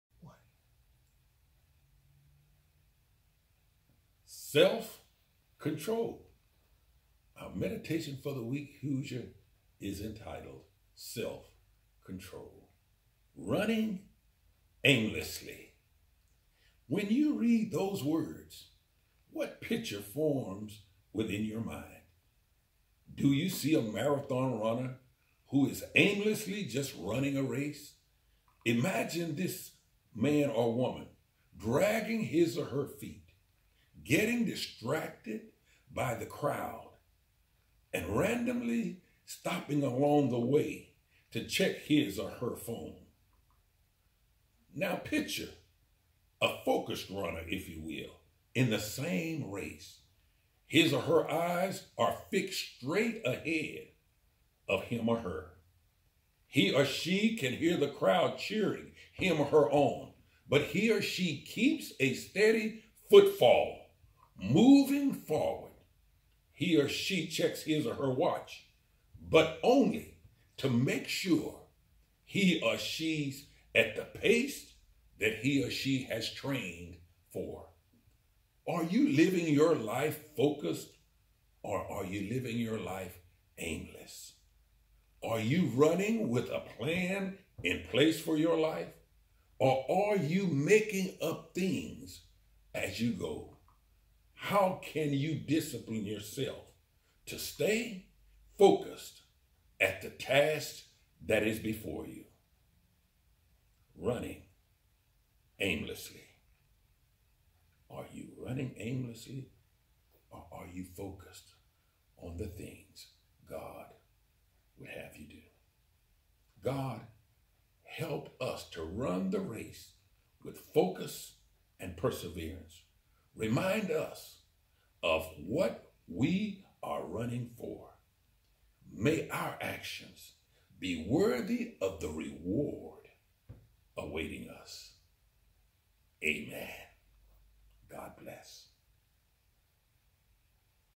Self Control
An Inspirational Devotion by Pastor Gary D. Dean, senior pastor of Hossier Memorial UMC.